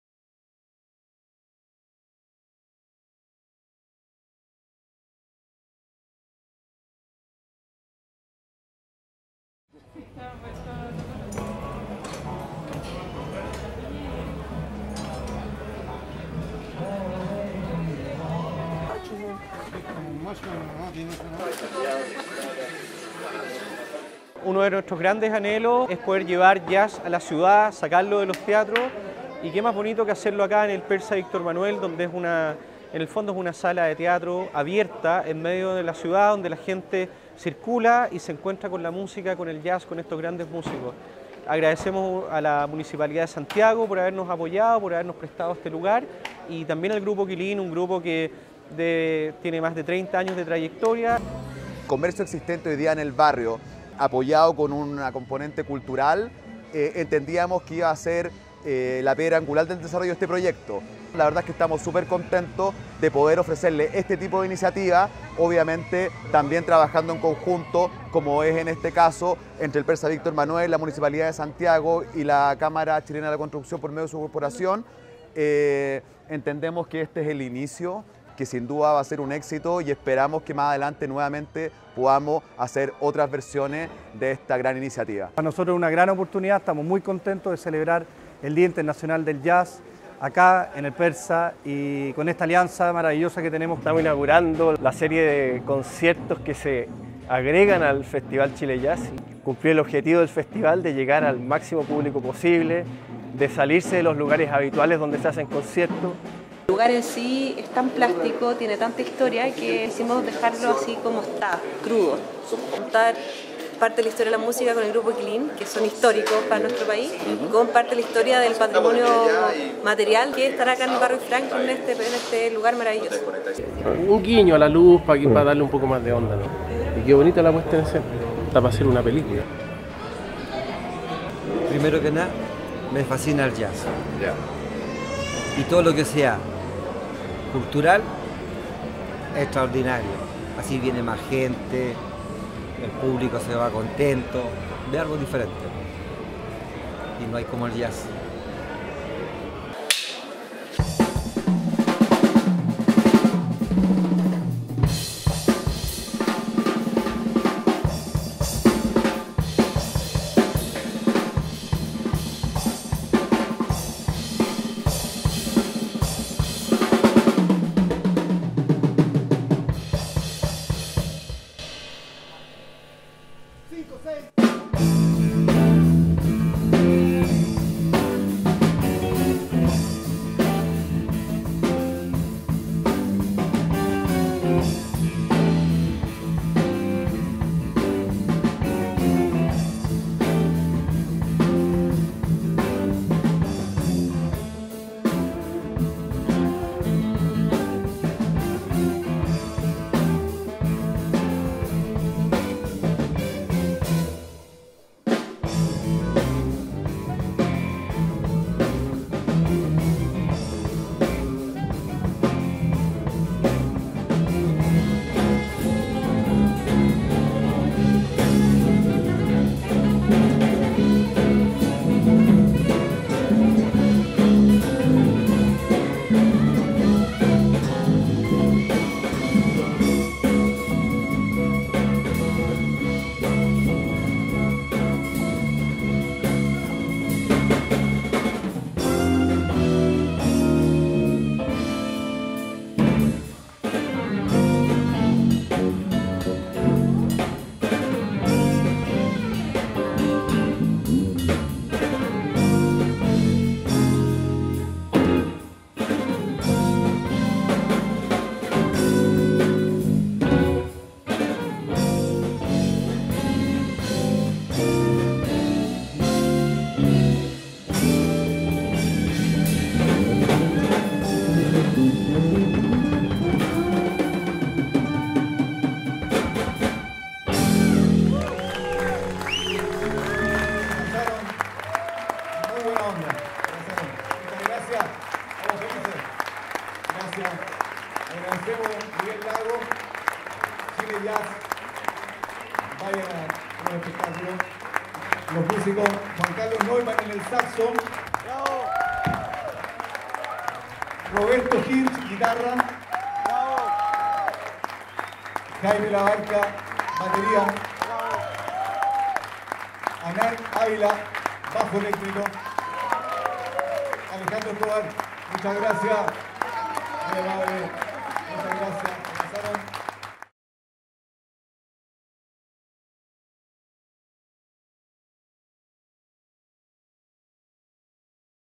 Festival Chile Jazz en Persa Bio Bio 2018
Intervención en el espacio público por Festival Chile Jazz en 2018